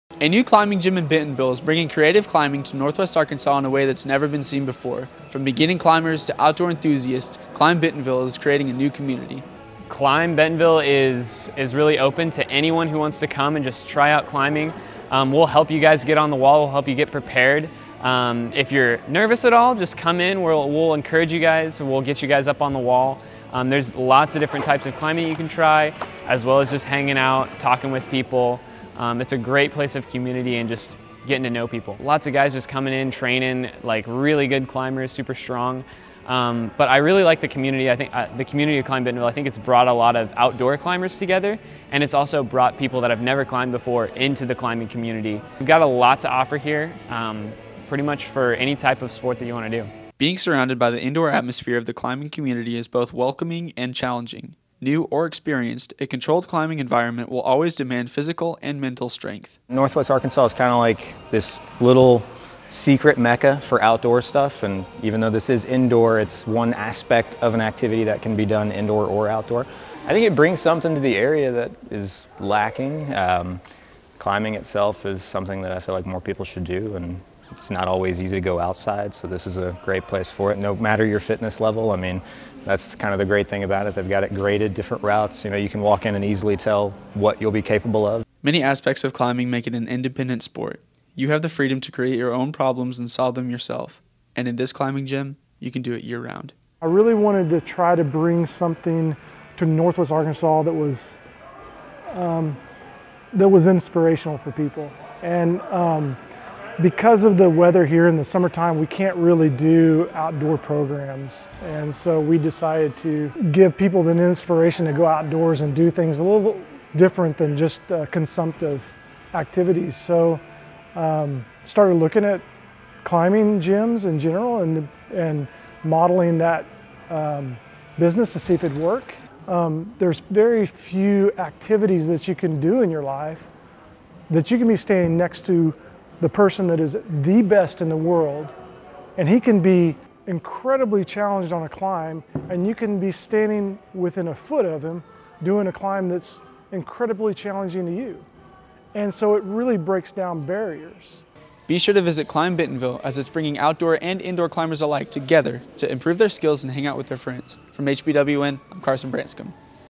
OMAF 2019 | Broadcast Human Interest/Feature Story | Climb Bentonville
Broadcast Human Interest/Feature Story | Climb Bentonville | Har-Ber High School | Carson Branscum, John Areelano, Chris Perea, and Alexsander Chavez